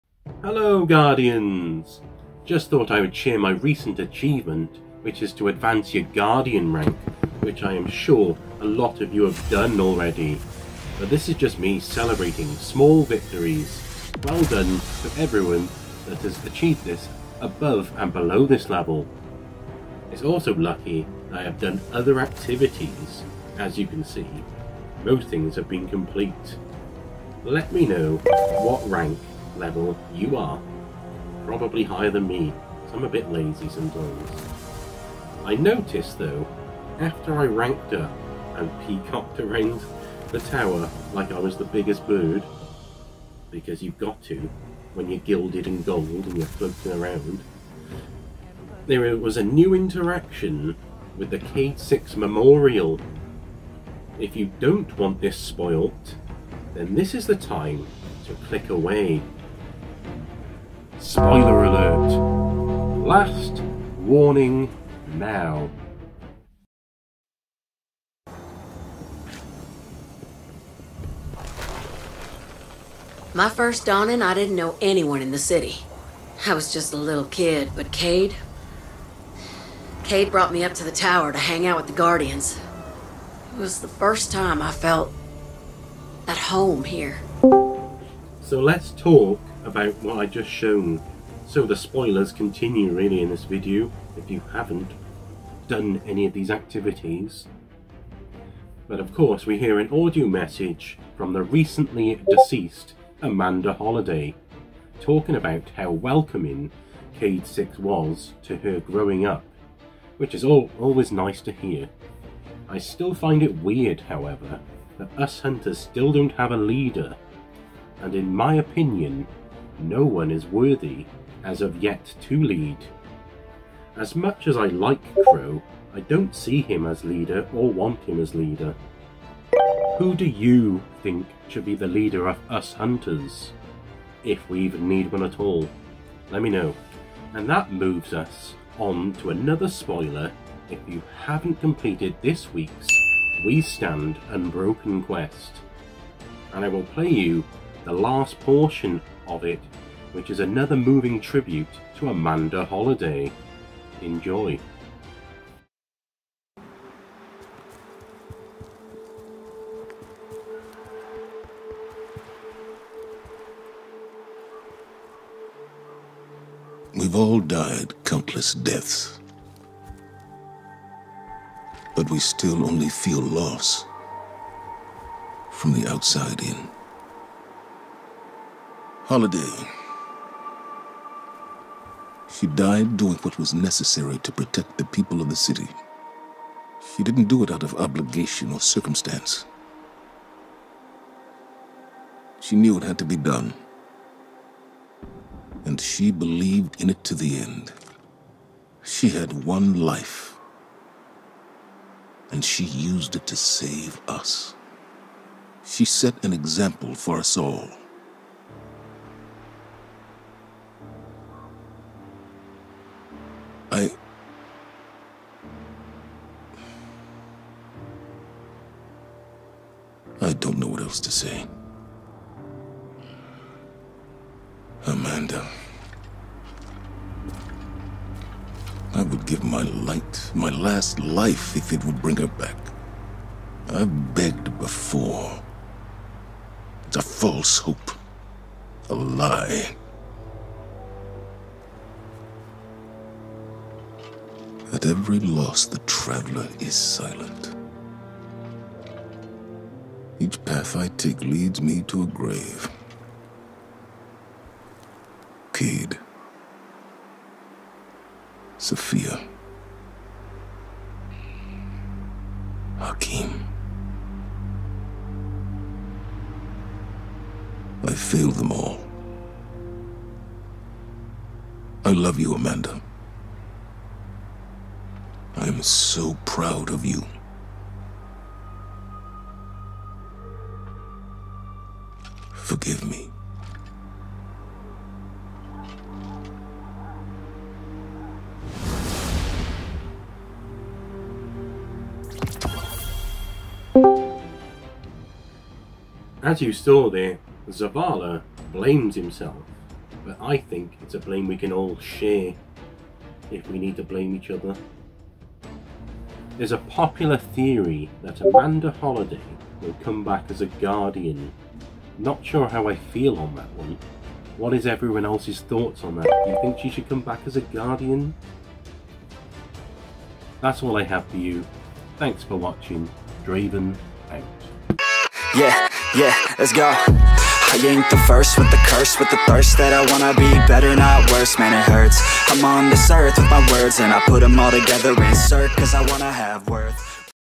We Stand Unbroken. (Let's Talk About This)
Lets talk about the recent Destiny 2 story quests "We stand unbroken" aswell as other topics. MAY CONTAIN SOME SPOILERS

epicpartner Use my code AGENTDRAVEN in the item shop✔️

Equipment used:
Clip Champ
FiFine Microphone